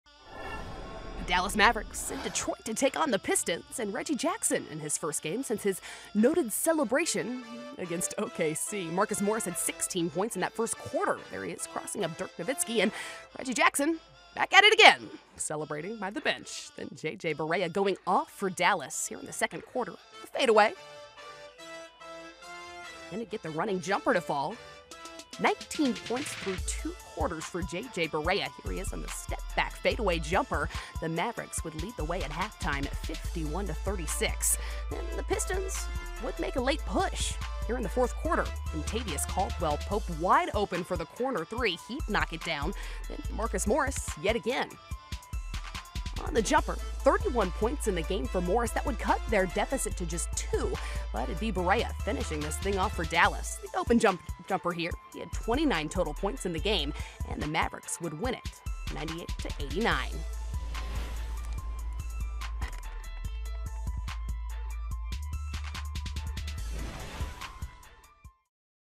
Dallas Mavericks vs Detroit Pistons - April 1, 2016
JJ Barea scores 29 points as the Mavericks defeat the Pistons 98-89.